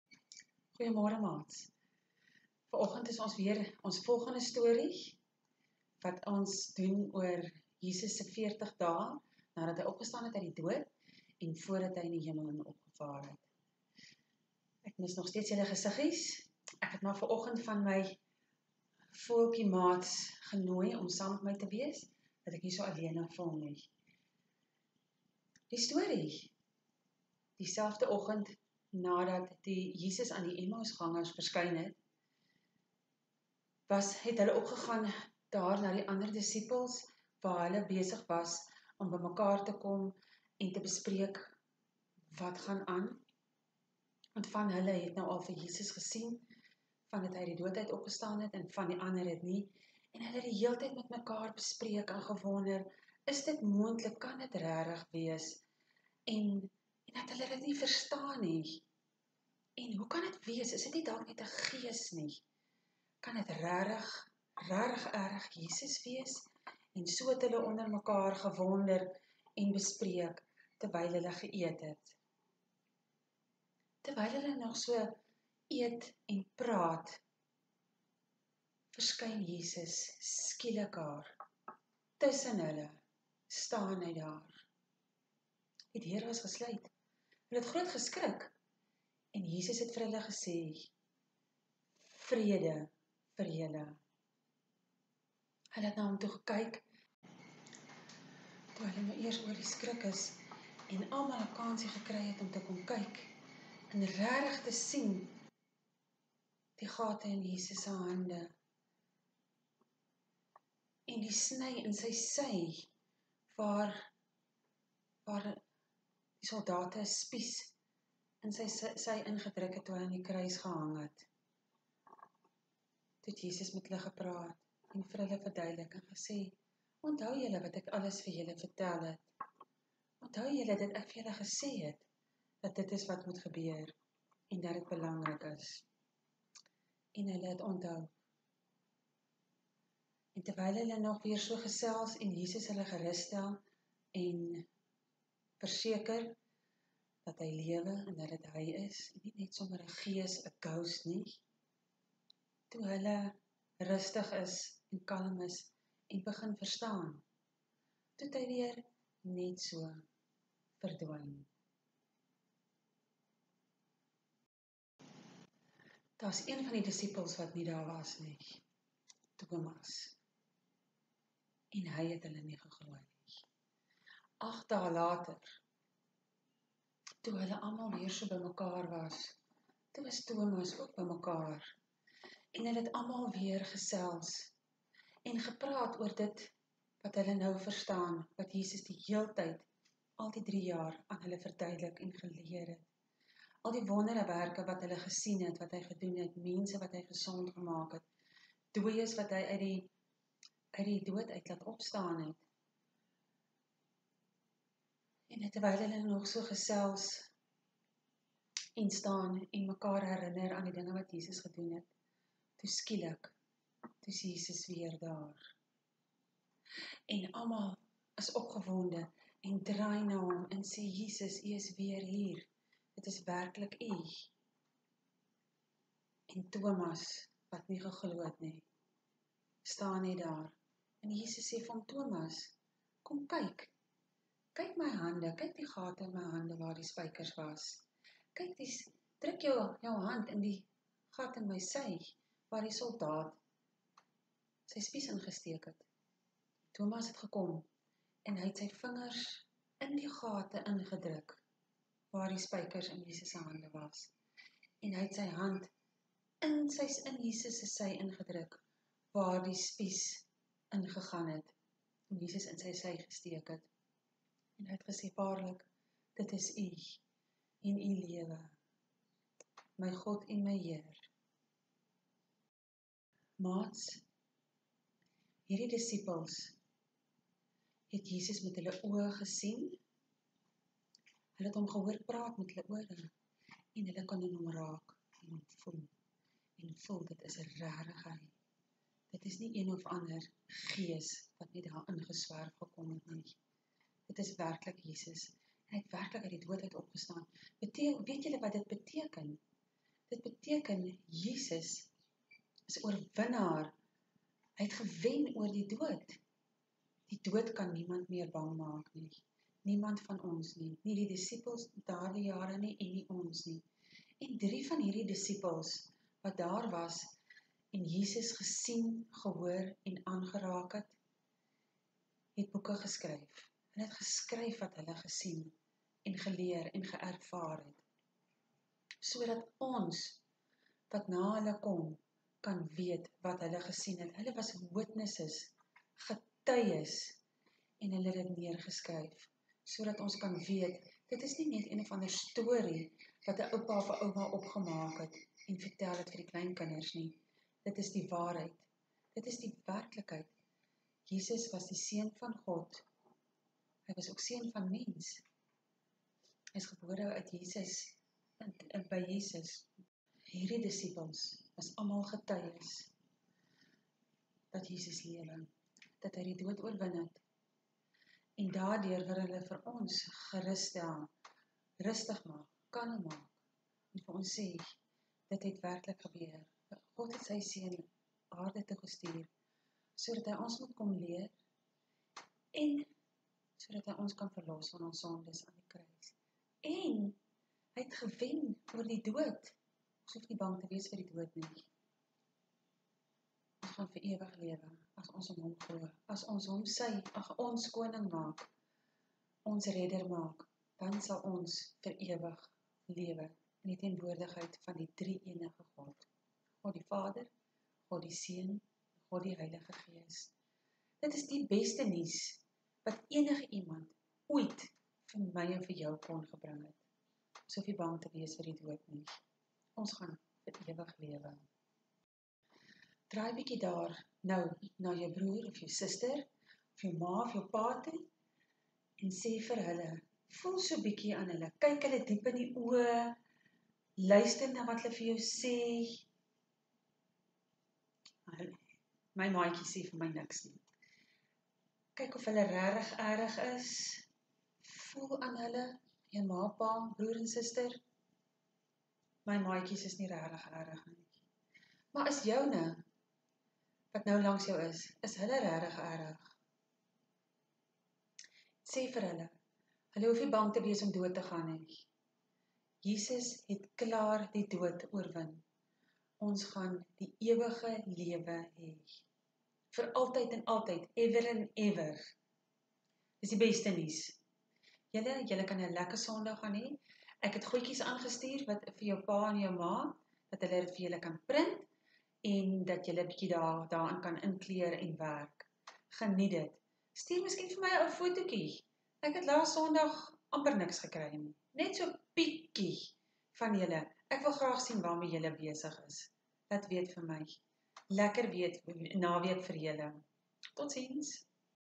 DitKerk Les 3 Mei 2020
Skakel in by Tannie Carmen, en leer saam oor die verhaal tussen Jesus en Tomas.